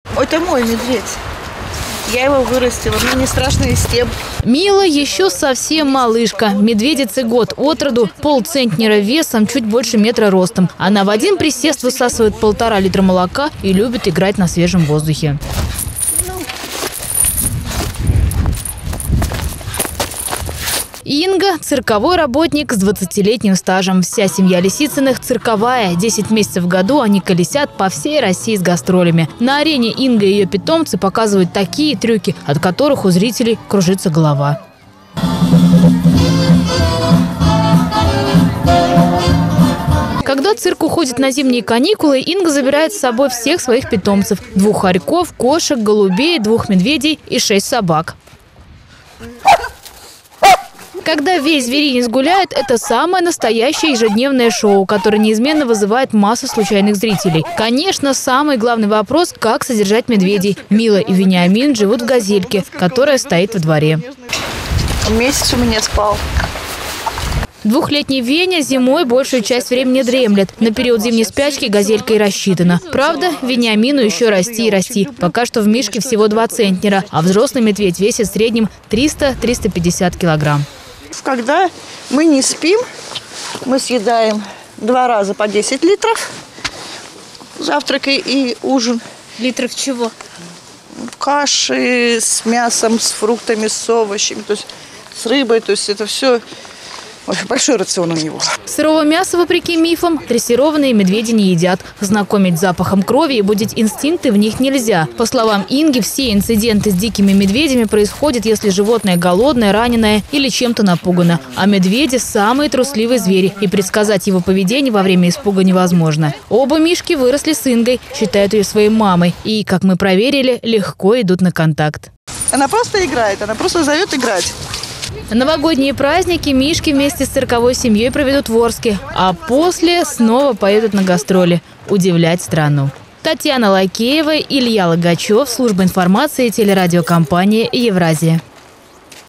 ЛАСКОВЫЙ И НЕЖНЫЙ ЗВЕРЬ
В одном из дворов Орска поселился опасный хищник. Наши  коллеги решили выяснить как такое возможно, приехали на место и убедились- бурый медведь действительно живет и не один, а в компании со своим собратом. Каков характер у косолапых и что они предпочитают на обед.